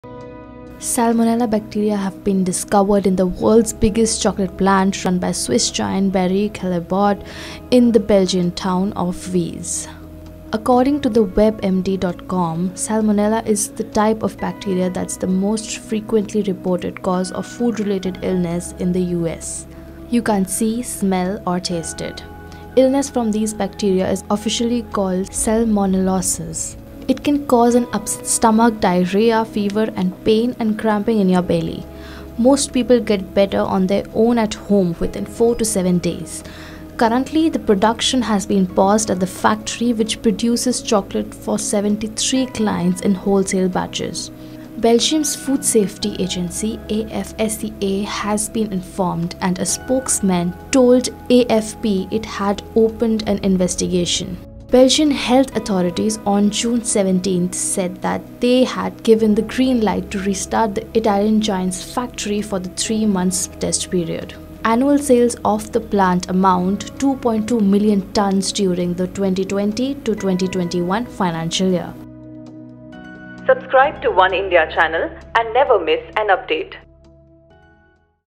Salmonella Bacteria found in world's biggest chocolate plant| OneIndia News*News
Salmonella bacteria have been found in the world's biggest chocolate plant,run by Swiss giant Barry Callebaut in the Belgian town of Wieze.